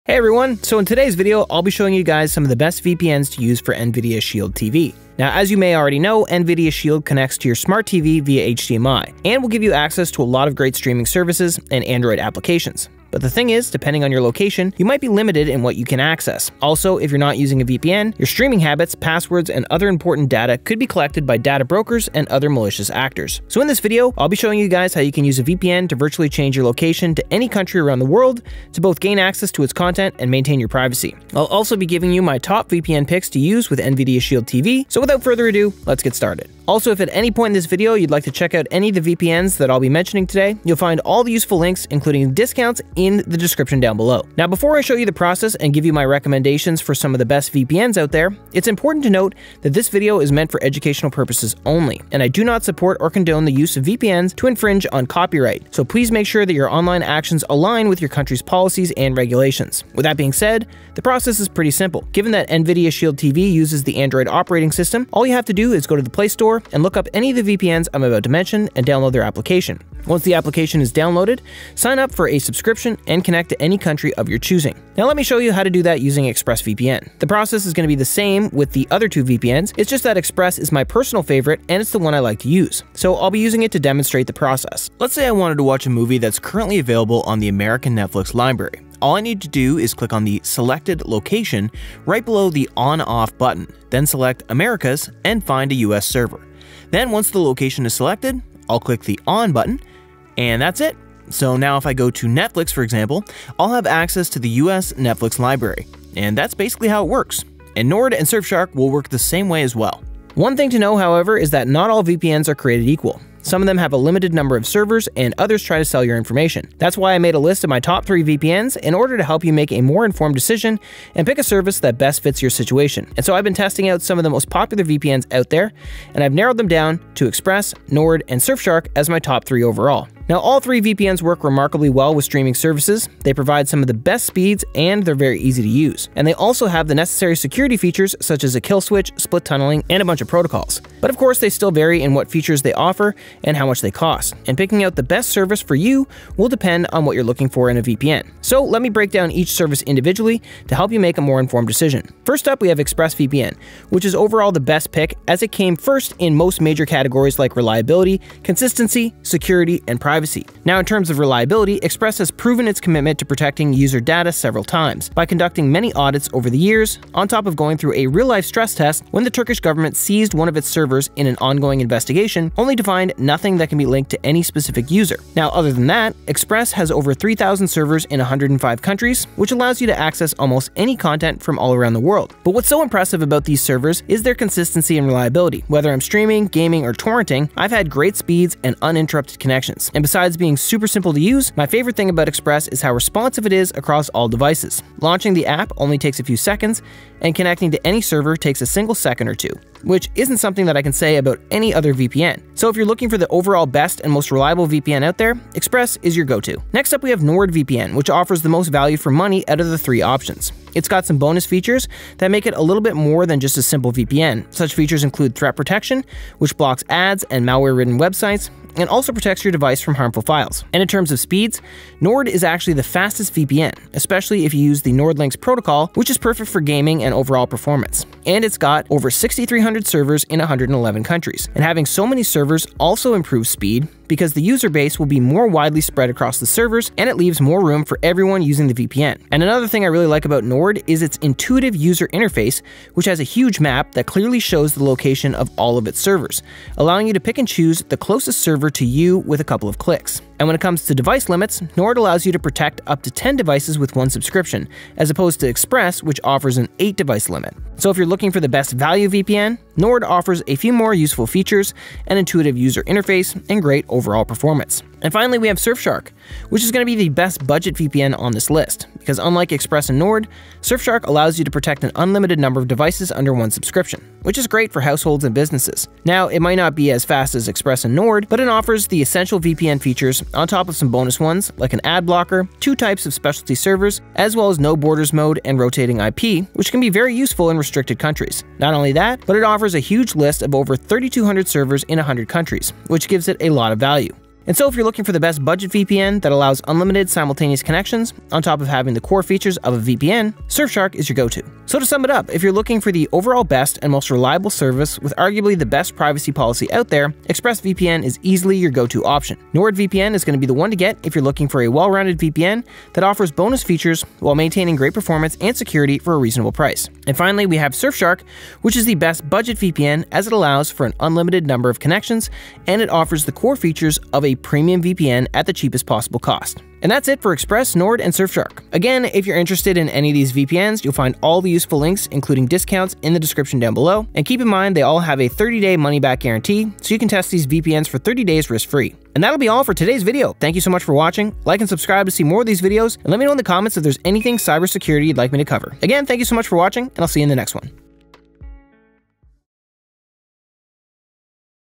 Best VPN For Nvidia Shield TV: Quick Setup without Issue
🌐Best VPN For Nvidia Shield TV🌐

Introduction and Importance of VPNs for Nvidia Shield TV

Welcome back, everyone! Today's video focuses on enhancing your Nvidia Shield TV experience with VPNs. Nvidia Shield TV offers access to numerous streaming services and applications, but regional limitations may restrict your content access.

Moreover, without a VPN, your online activities could be vulnerable to data collection by malicious actors. Therefore, I'll guide you through the process of using a VPN to change your virtual location, enabling access to global content while safeguarding your privacy.

How to Use VPNs with Nvidia Shield TV

Using VPNs with Nvidia Shield TV is straightforward, thanks to its Android operating system. Simply navigate to the Play Store, search for your preferred VPN, and download its application. After signing up for a subscription, connect to a server in your desired country. For illustration, I'll demonstrate with ExpressVPN.

Suppose you wish to access Netflix UK. Just select the UK server from the available options, and you're all set! However, not all VPNs offer the same quality of service, so I've curated a list of the top three VPNs to aid your decision-making process.

Top VPN Picks and Conclusion

Now, let's dive into my top VPN recommendations for Nvidia Shield TV. ExpressVPN, NordVPN, and Surfshark stand out for their reliability, security features, and global server coverage. ExpressVPN excels in speed and ease of use, making it ideal for seamless streaming experiences. NordVPN offers robust security features and a vast server network, catering to diverse user needs.

Meanwhile, Surfshark provides excellent value for money with unlimited device connections and advanced security features. Remember, your online privacy matters, so choose your VPN wisely!

Hope you enjoyed my Best VPN For Nvidia Shield TV: Quick Setup without Issue Video.